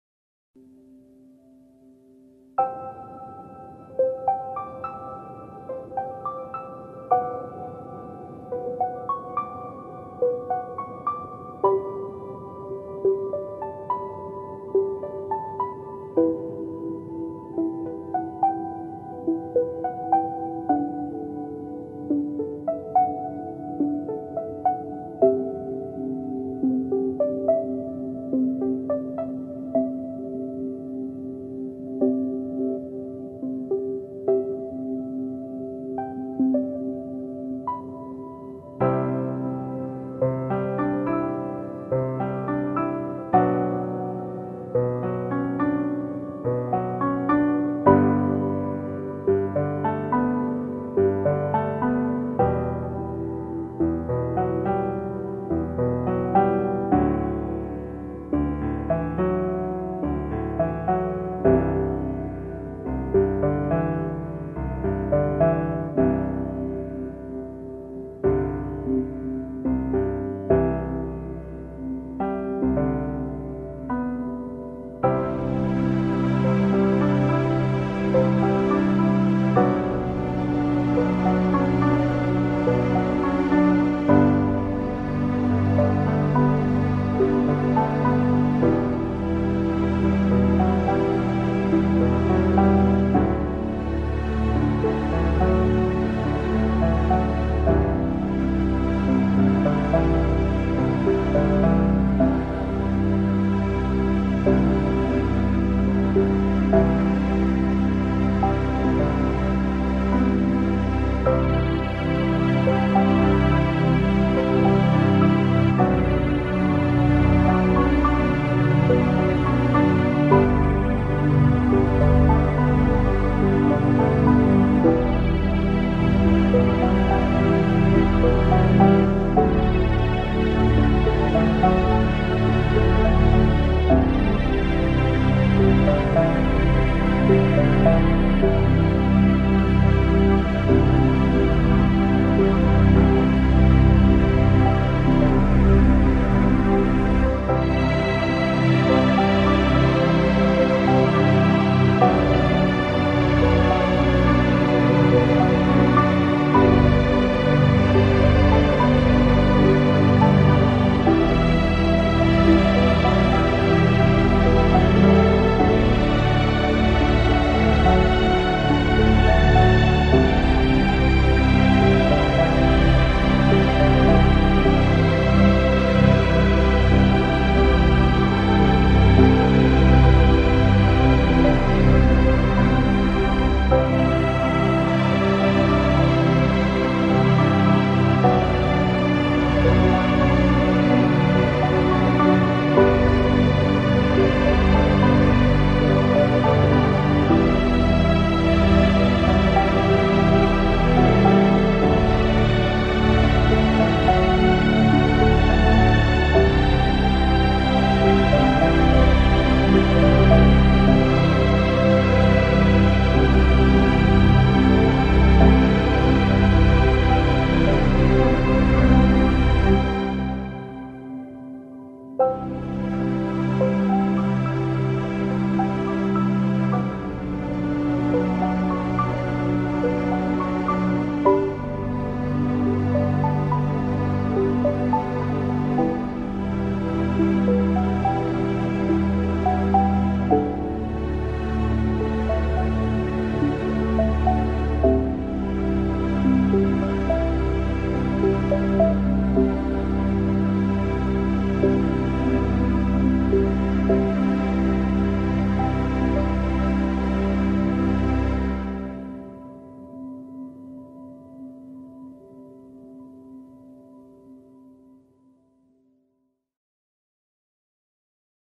BNSF coal empty HO Scale
BNSF coal empty on my layout running in DPU.  I've seen hundreds of videos of fast paced music and lots of action sounds, time to sit back and relax a little...  HO Scale